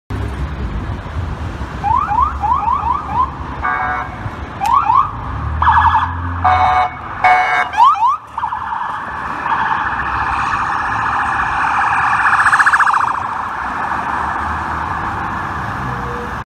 2 PATERSON POLICE DEPARTMENT CRUISERS RESPONDING TO SHOTS FIRED IN PATERSON, NEW JERSEY.
HERE YOU WILL SEE 2 PATERSON POLICE DEPARTMENT CRUISERS RESPONDING TO SHOTS FIRED IN PATERSON, NEW JERSEY.